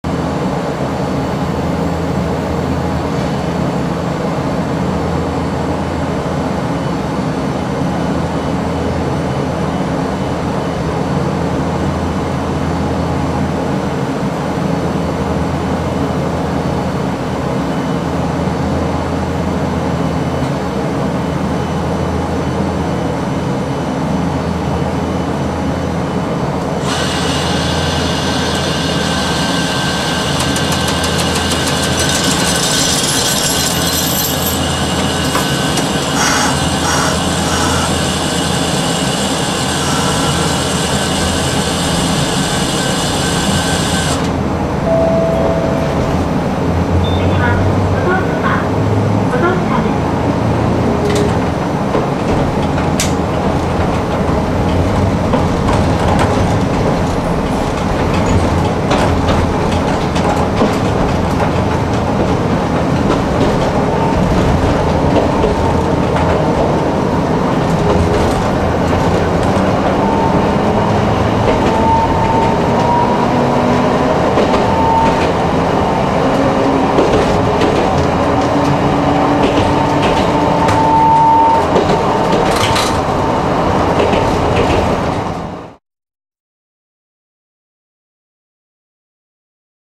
名鉄美濃町線 制限5km/h区間
名鉄美濃町線　制限5km/h区間　発車後すぐの踏切動作完了まで制限5km/hで走行。